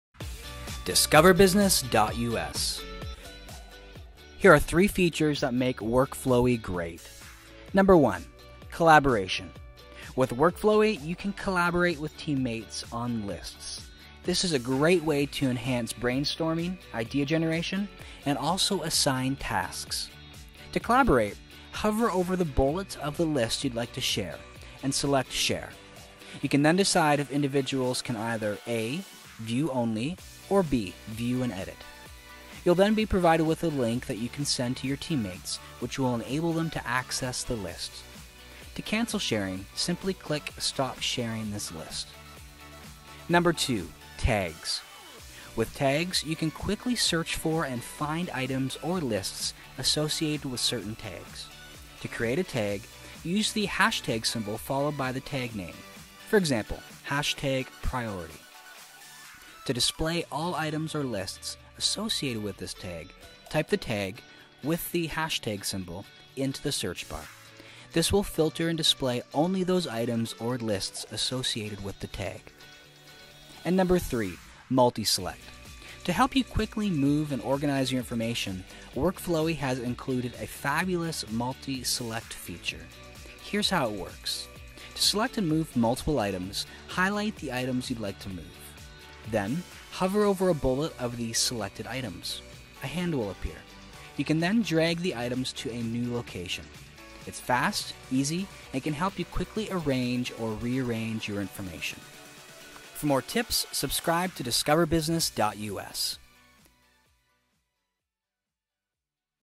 3 Features That Make Workflowy Great
Workflowy is great for making lists. In this video, learn about 3 basic yet great features that Workflowy has to offer.

This tutorial is part of our Guide to Effective Learning Series.

Transcript:

How to in 60 seconds or less -  Discoverbusiness.us

Here are 3 Features That Make Workflowy Great

1. Collaboration:

With Workflowy, you can collaborate with teammates on lists. This is a great way to enhance brainstorming and idea generation, and also assign tasks.

To collaborate, hover over the bullet of the list you’d like to share, and select share.
You can decide if individuals can either A) Only view the list or B) View and Edit.

You’ll then be provided a link that you can send to your teammates, which will enable them to access the list. To cancel sharing, simply click “stop sharing this list”.

2. Tags:

With tags, you can quickly search for and find items or lists associated with certain tags. To create a tag, use the # symbol followed by the tag name, for example, priority.

To display all items or lists associated with this tag, type the tag (with the# symbol), into the search bar. This will filter and display only those items or lists associated with the tag.

3. Multi-Select:

To help you quickly move and organize your information, Workflowy has included a fabulous multi-select feature. Here’s how it works:

To select and move multiple items, highlight the items you’d like to move. Then, hover over a bullet of the selected items. A hand will appear. You can then drag the items to a new location.

It’s fast, easy, and can help you quickly arraange or re-arrange your information.